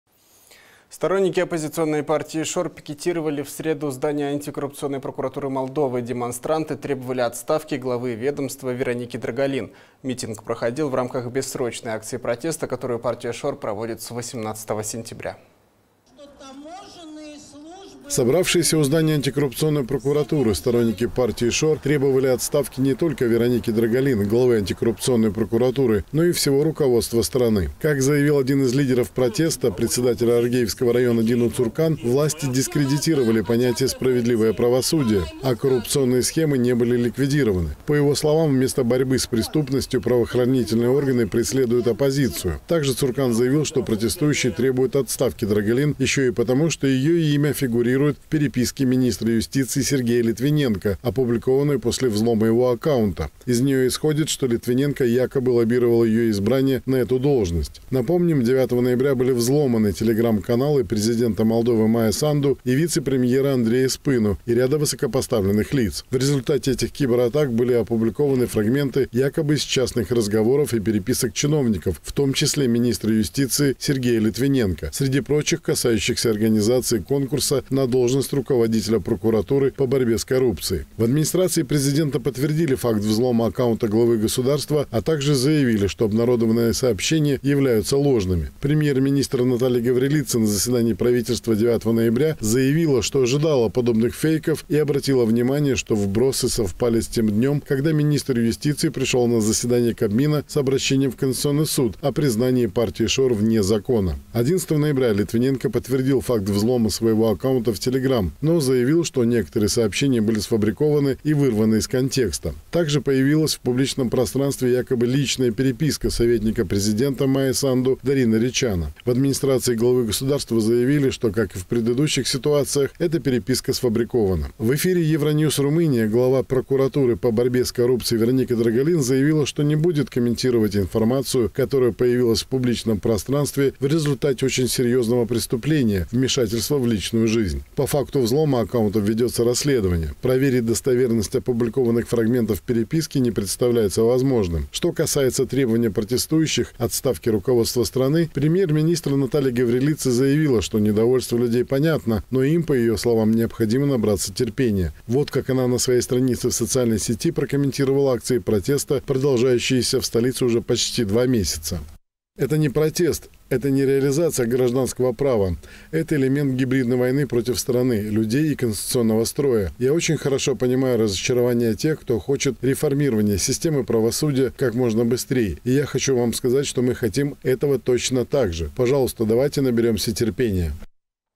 Требуют отставки
Сторонники оппозиционной партии "Шор" пикетировали во вторник здание антикоррупционной прокуратуры Молдовы. Митингующие требовали отставки главы ведомства Вероники Драгалин. Митинг проходил в рамках бессрочной акции протеста, которую партия «Шор» проводит с 18 сентября.